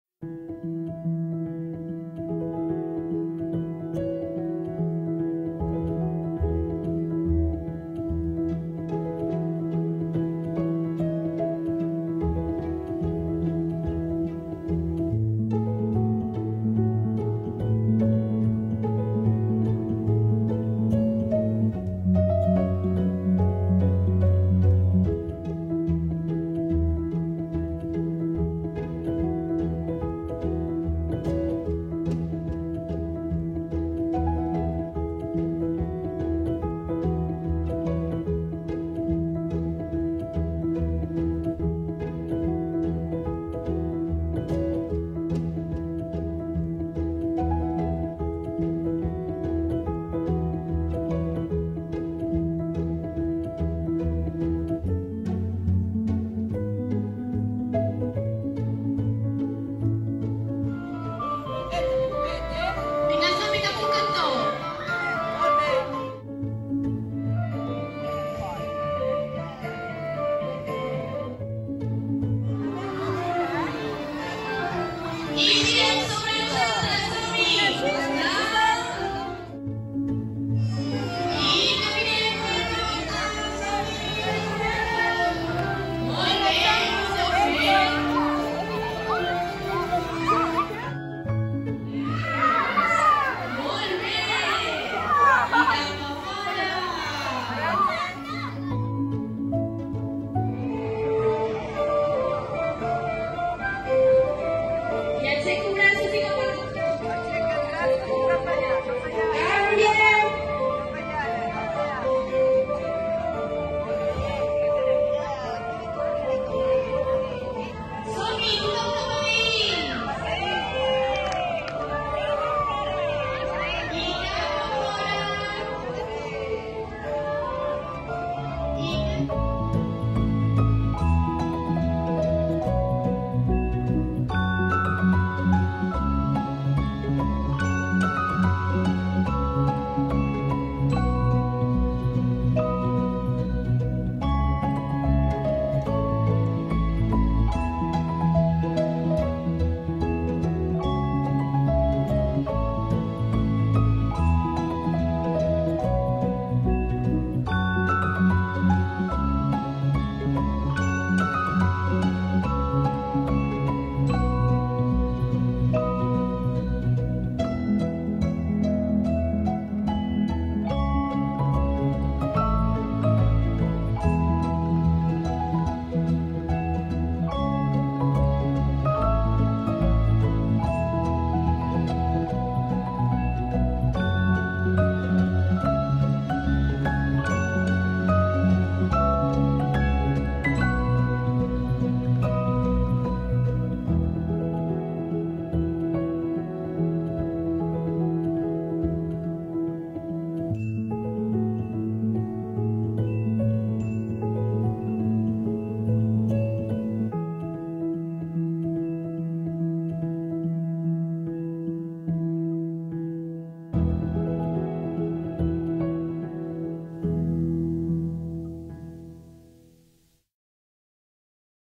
Un viatge a l'Edat Mitjana
Hem acabat el curs i per celebrar-ho hem fet un viatge molt especial a l'Edat Mitjana.